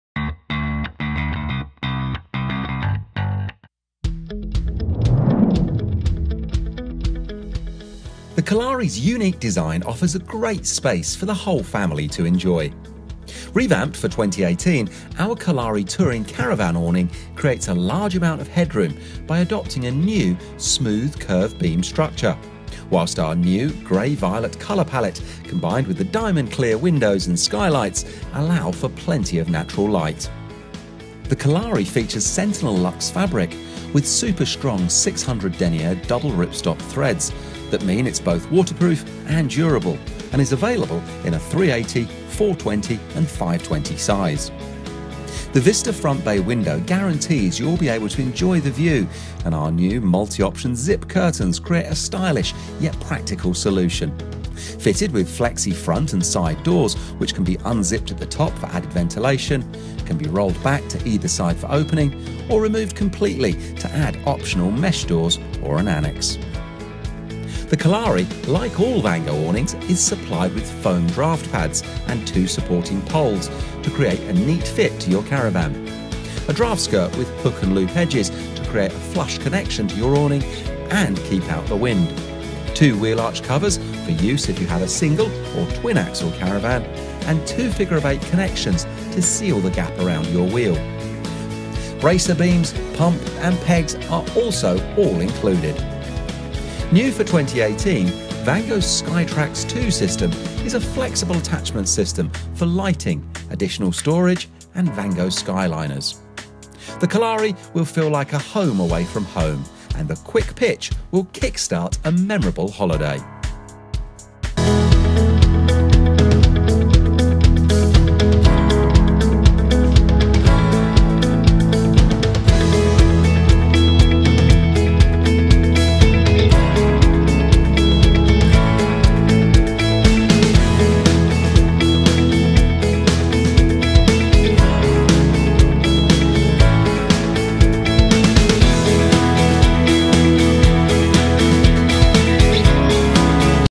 Vango Kalari 380, 420 & 520 Awning 2018 Review
Vango Kalari is Vango's flagship awning with the best material 600D luxe with super beams and all the top features

click our website link for more info and prices or call for package deals